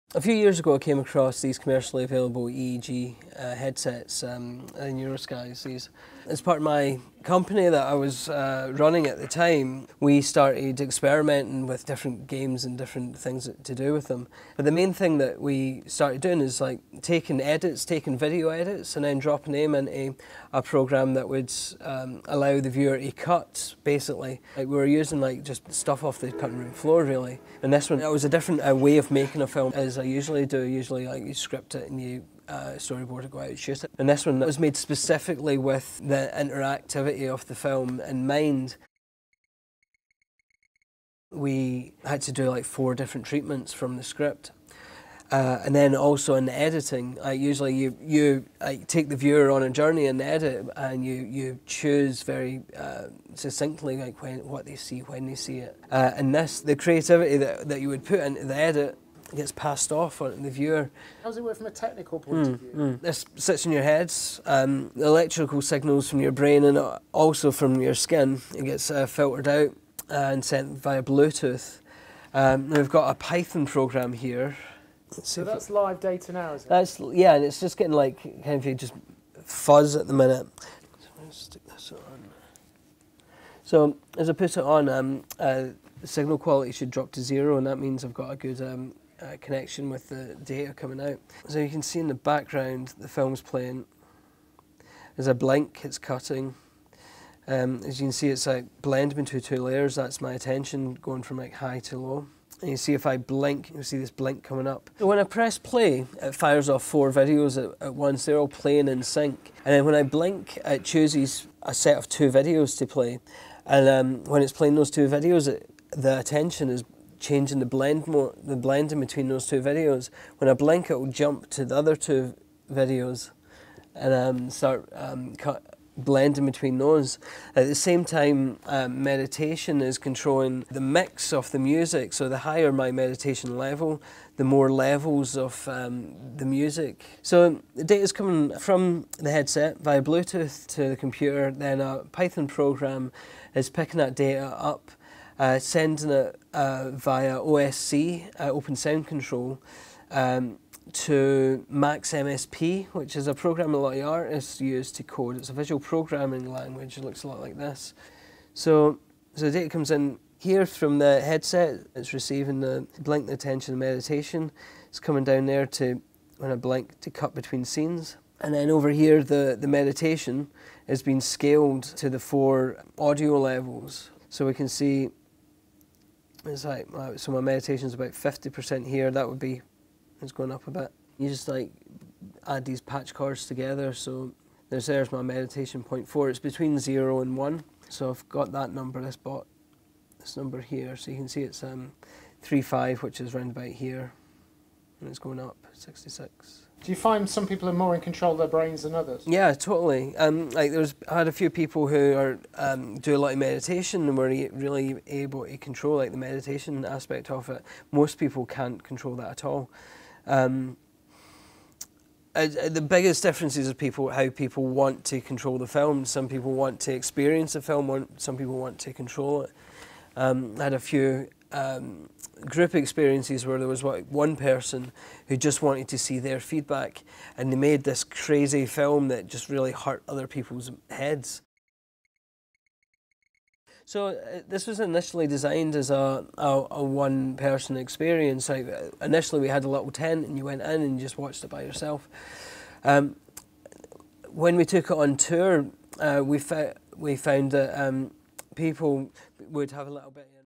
Brain Controlled Movie - Computerphile
A movie where you're deciding the edits, consciously and subconsciously. Richard Ramchurn and his brain controlled movies.